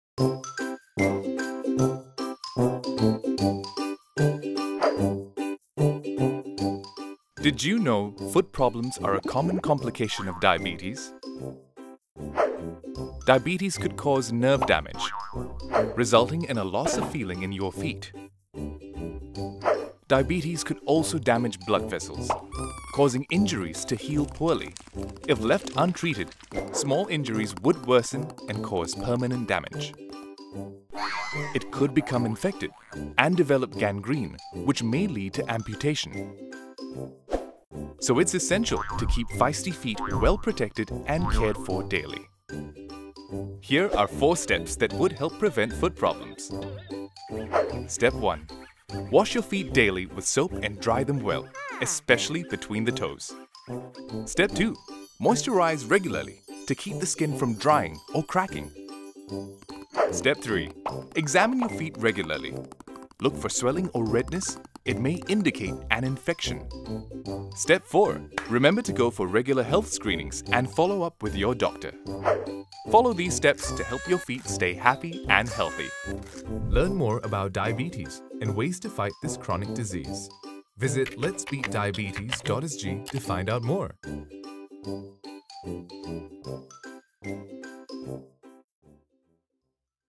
Organs Family & Diabetes: Feisty Feet
Myth or fact: Diabetes results in foot problems.
Watch this video for the answer.

to learn more about how you can keep diabetes at bay.